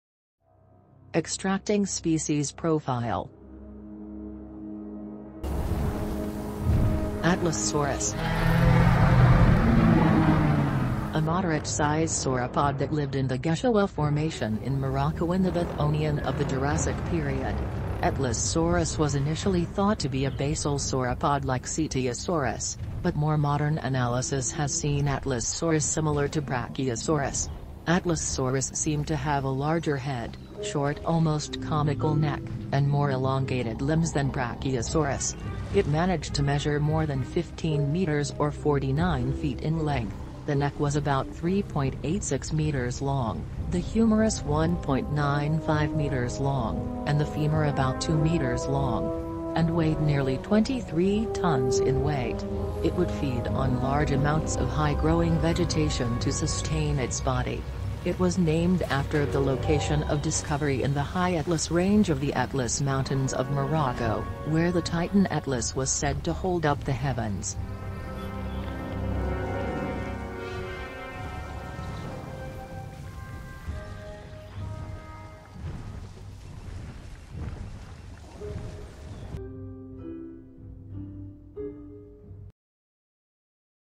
Atlasaurus Species Profile
Atlasaurus species profile. Alongside Brachytrachelopan, this sauropod has another short neck